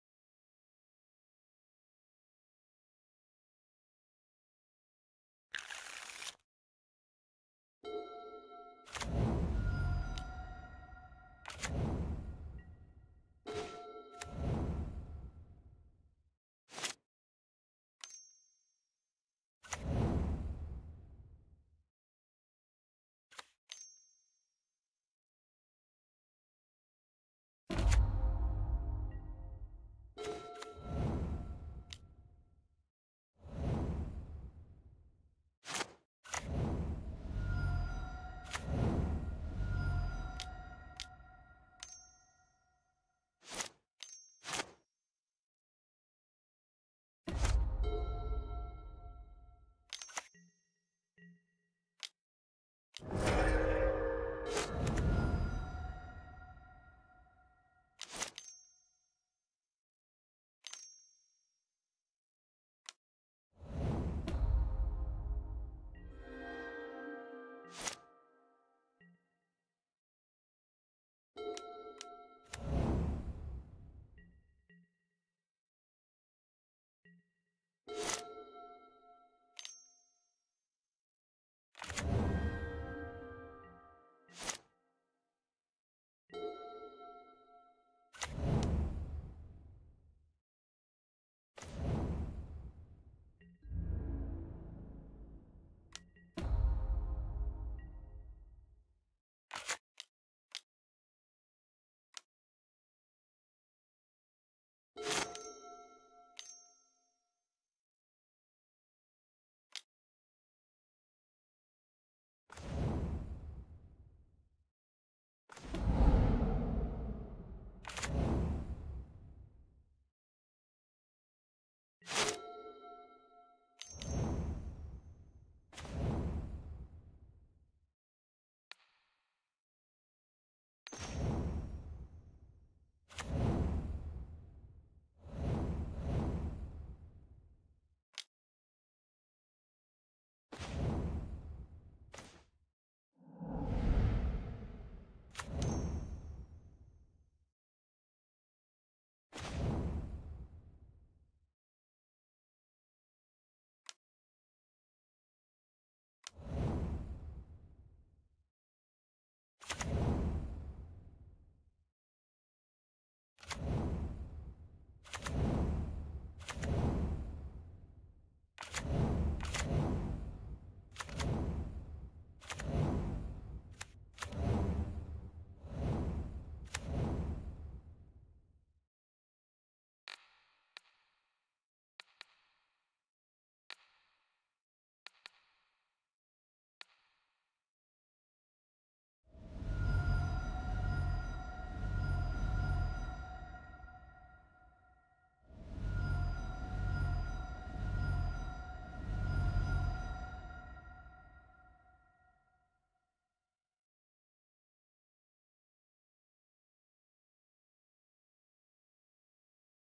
PAENG4983 MODERN PRELIMINARY round 5 part 3 of 3 FETCHLESS STORM vs FELIDAR COMBO OMNATH 13 Oct 2020
I waited for the perfect moment to go off.

That is when he is low on resources (mana).

Also, be aware that you'll need to dig deeper for those combo pieces.

Be reminded that you can counter your own spell with Remand and dig a little deeper with Baral's trigger.